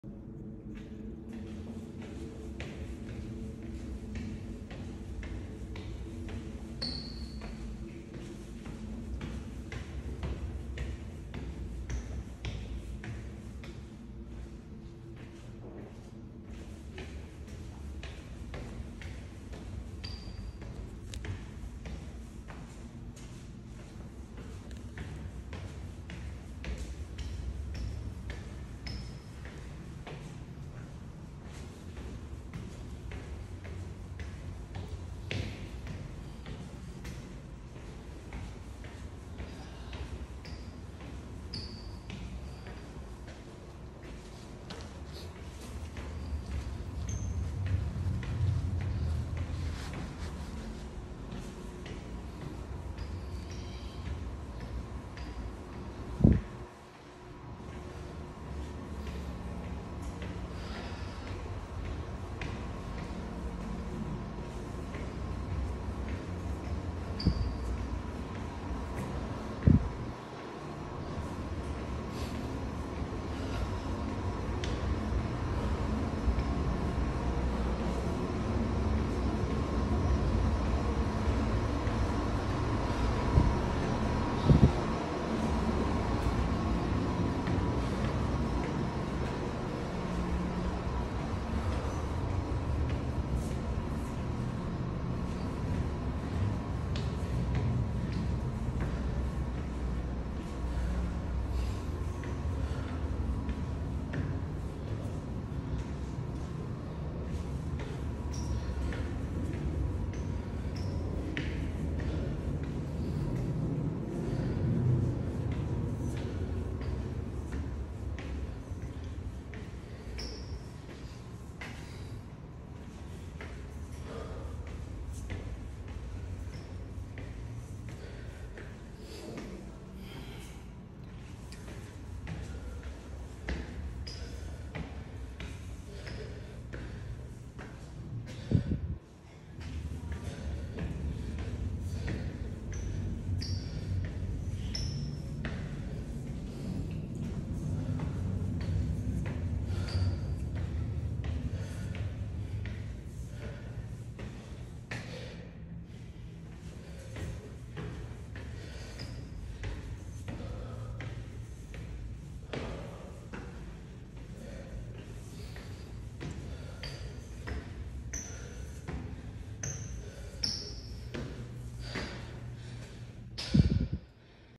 ASMR Walking Up Stairs 1-12 Floors 240 Steps 2.55 mins Challenge None-Stop 10/13/2022
Walking Up Stairs 1-12 Floors 240 Steps 2.55 mins Challenge None-Stop
Next challenge video: